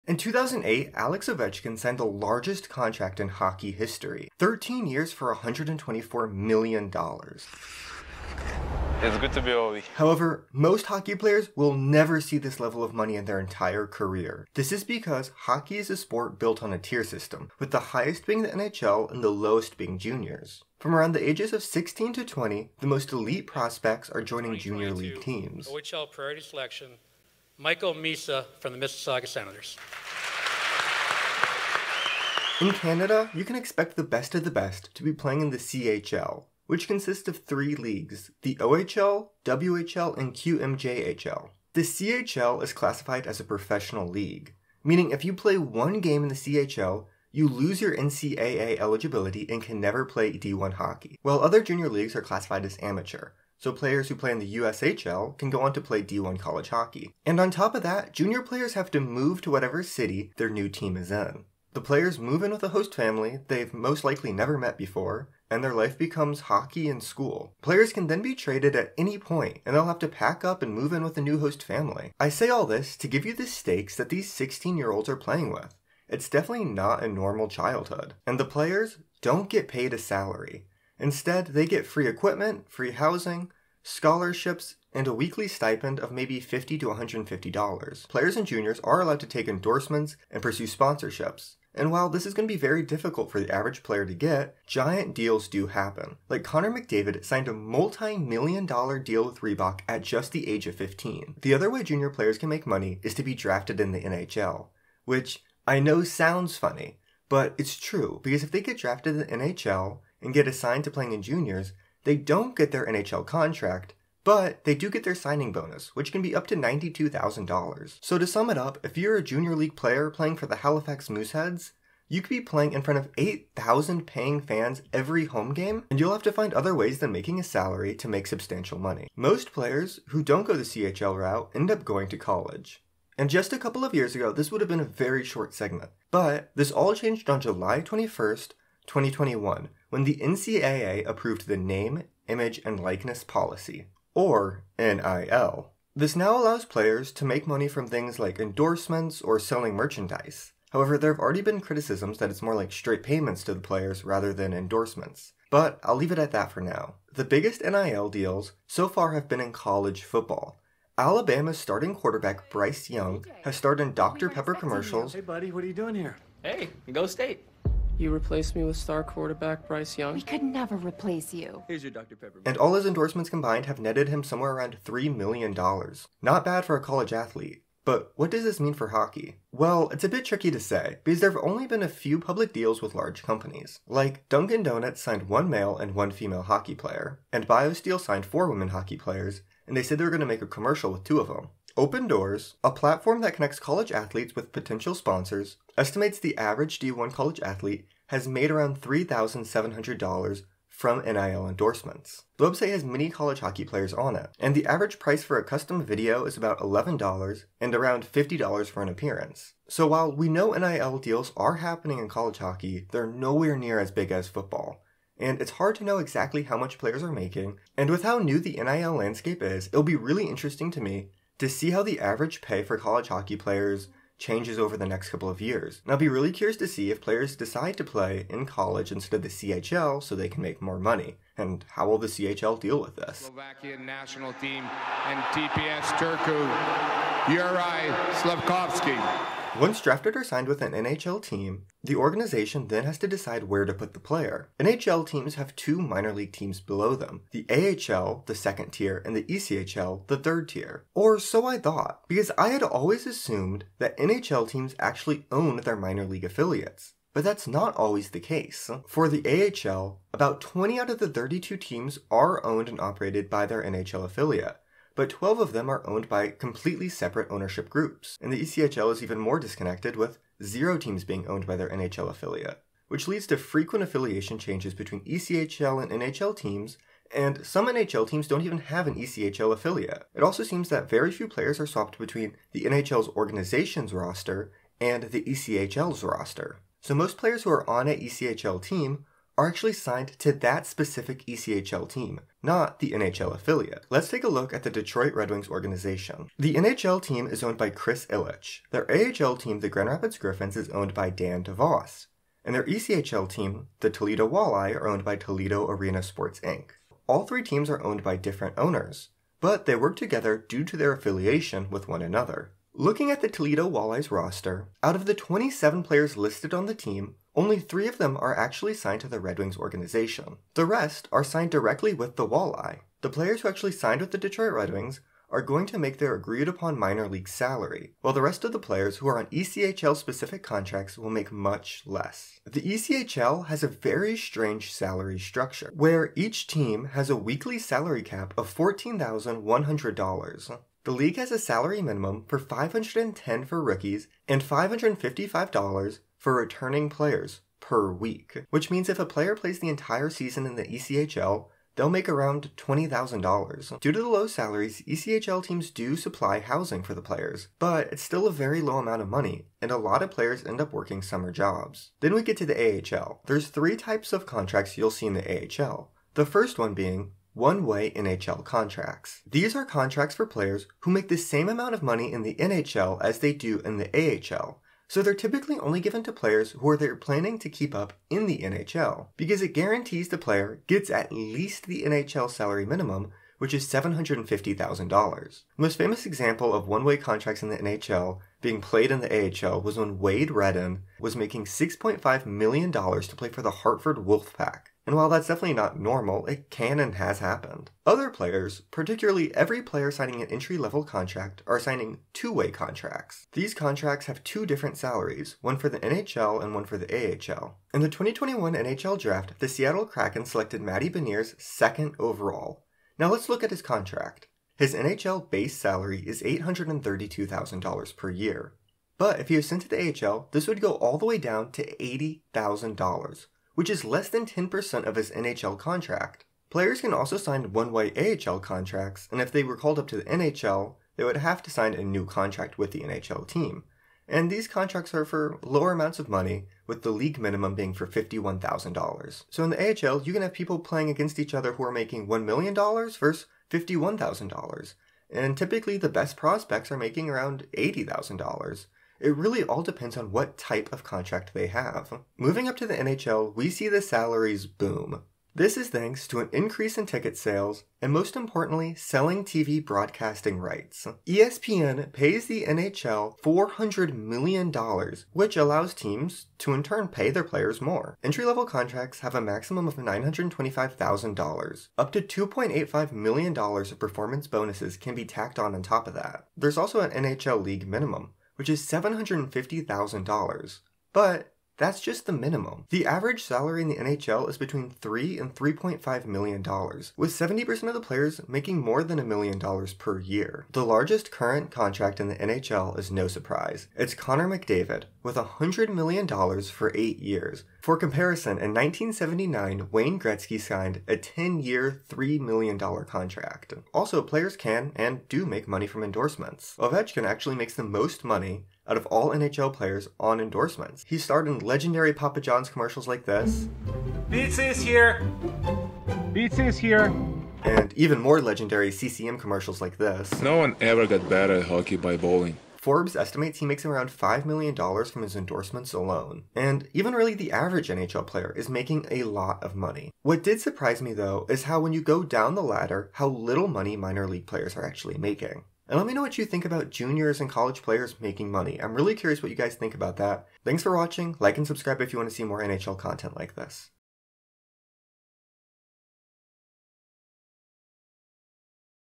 How Much Money do Hockey Players Make?
How much money do hockey players actually make? Its a good question because there are many leagues in hockey. From the OHL, QMJHL, WHL, College hockey, ECHL, AHL, to the NHL there is vastly different amount of money being paid to players. NHL players, like Connor McDavid and Alex Ovechkin, are earning a very large amount of money especially with sponsorships and performance bonuses. But players in Juniors like Connor Bedard are making no salary from their team. With the introduction of NIL deals the amount that college players are making is changing rapidly. Let me know what you think will happen with college and juniors pay moving forwards.